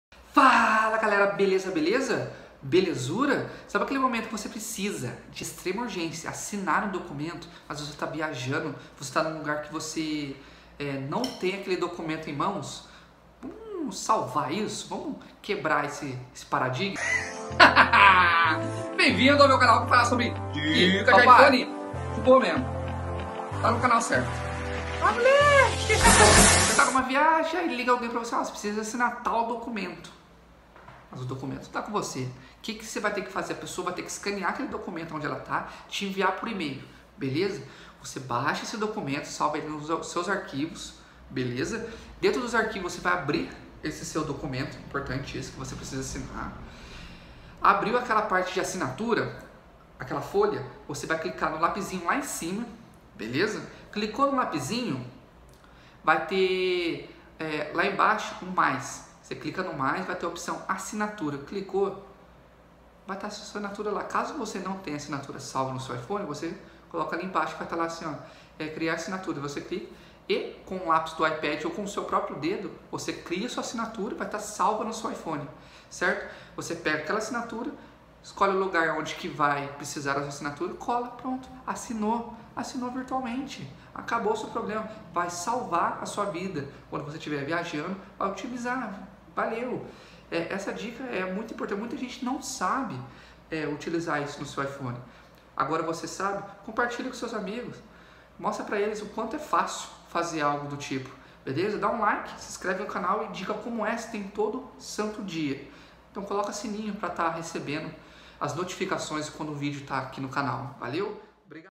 COMO ASSINAR DOCUMENTOS NO IPHONE ?
Você sabia que pode assinar documento pelo seu iPhone ?🖊

Imagine que você esteja numa viagem e precise assinar um documento.  O que  você faria ?

Acompanhe o vídeo e descubra como fazer uma assinatura pelo seu iPhone.